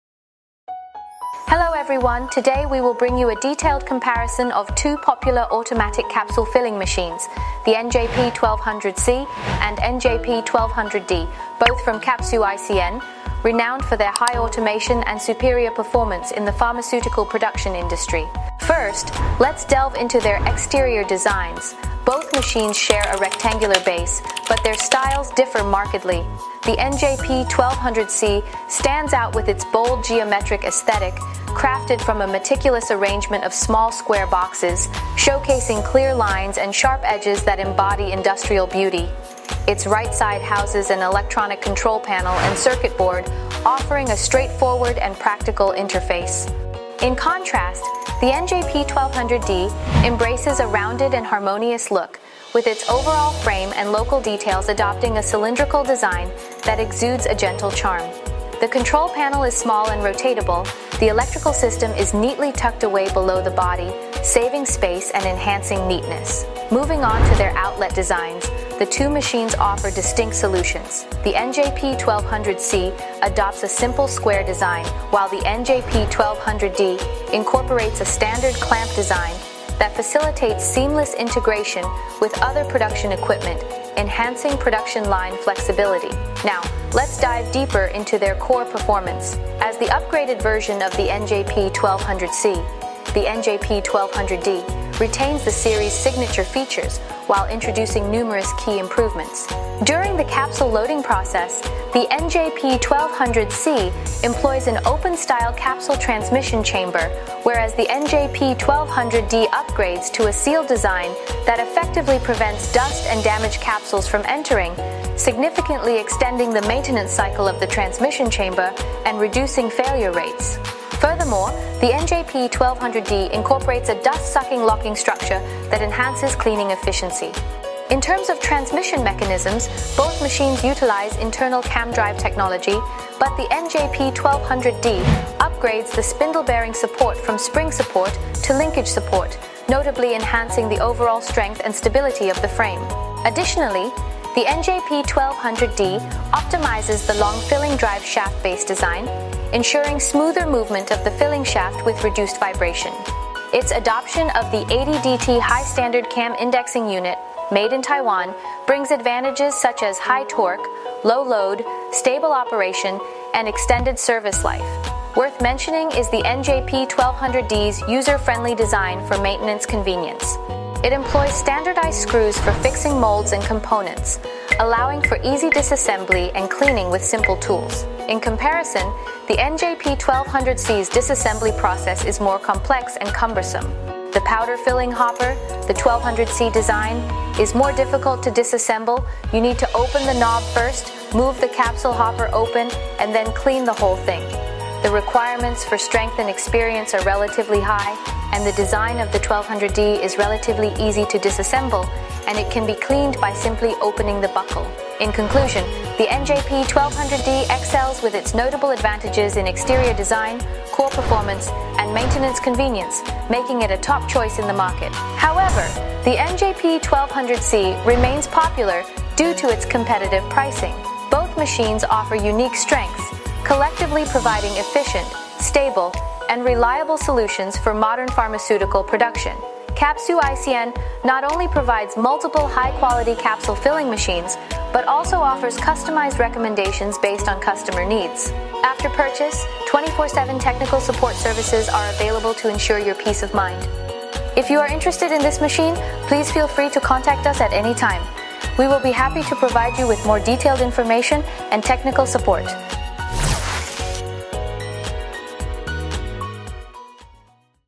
🌟【Pharmaceutical Machinery Competition】NJP-1200C vs NJP-1200D🌟
Hello everyone👋! Welcome to today's video, where we will take an in-depth look at two very popular automatic capsule filling machines - NJP-1200C and NJP-1200D models from CapsuICN!

🔍If you are looking for a solution that can improve production efficiency while maintaining high quality standards, then you must not miss this detailed product comparison. CapsuICN is well-known in the pharmaceutical production field for its high degree of automation and excellent performance, and these two models are one of its masterpieces.

👉In this video, we will carefully analyze the characteristics, advantages and possible application scenarios of each model to help you make the most appropriate choice based on your actual needs.